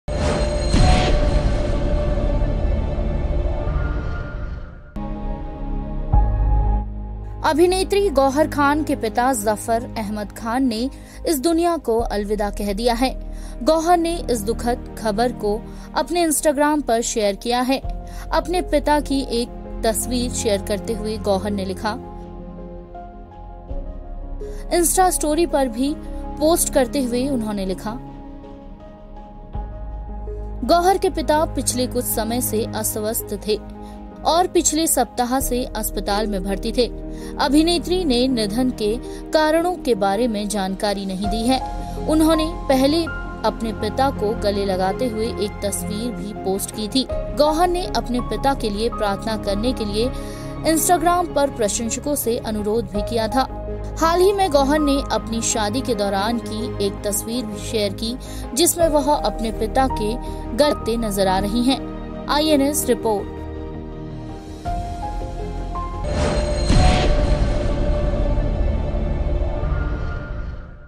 Gauahar Khan के पिता Zafar Ahmed Khan ने दुनिया को कहा अलविदा
अभिनेत्री गौहर खान के पिता जफर अहमद खान ने इस दुनिया को अलविदा कह दिया है। गौहर ने इस दुखद खबर को अपने इंस्टाग्राम पर शेयर किया है।